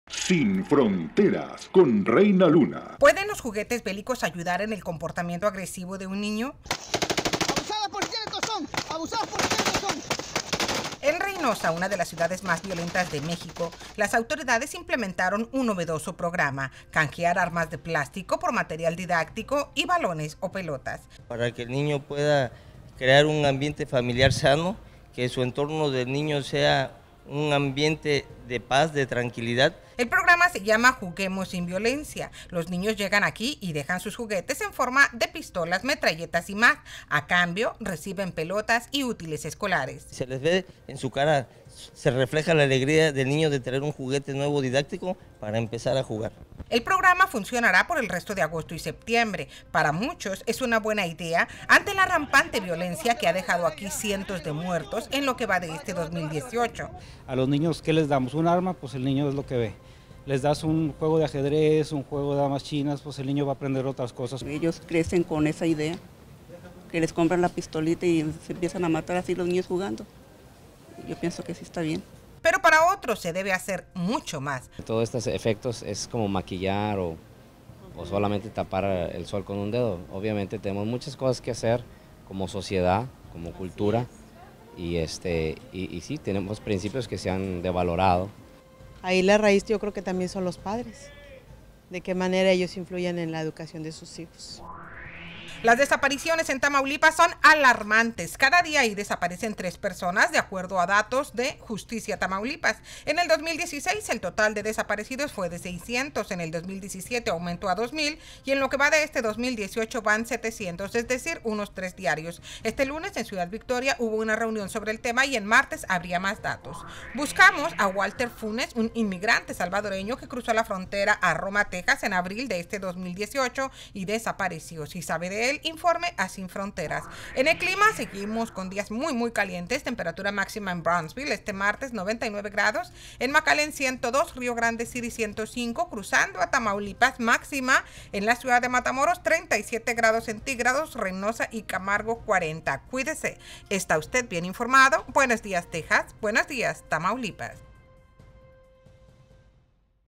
Cada día desaparecerían tres persona en Tamaulipas!!!
Canjean juguetes bélicos por juguetes didácticos, pero la población quiere más...En Estados Unidos ser hispano significa ganar menos que los sajones y puertorriqueños, pero si se es mujer se gana menos que sajonas, puertorriqueñas y cubanas!!! Buscamos a Walter Funes, desaparecido en Texas... Días de calor como en canícula!